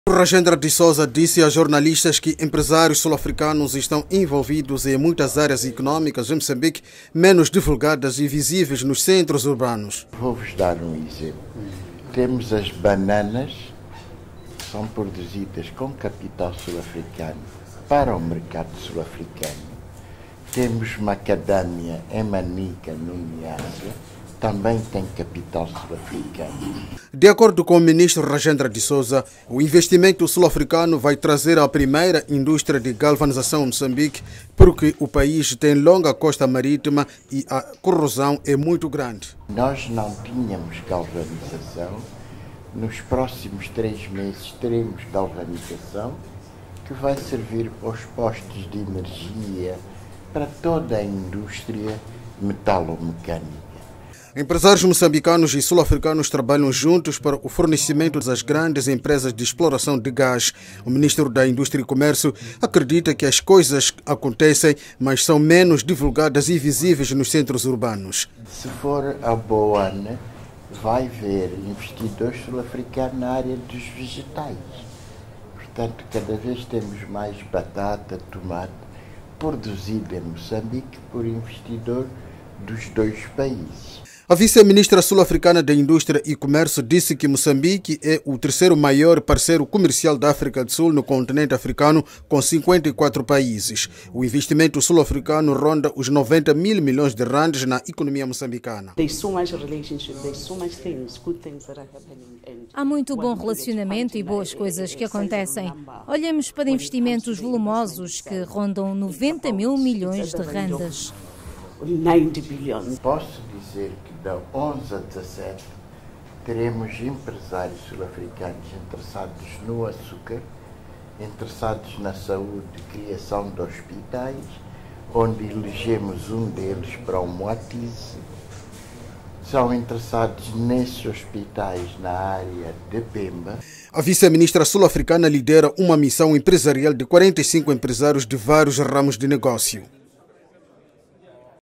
Moçambique vai ter novos investimentos de empresários sul-africanos
Empresários sul-africanos vão trazer a Moçambique nos próximos três meses a primeira industria de galvanização que pode ser usada para a protecção de postes de transporte de energia eléctrica e produtos metalo-mecânicos.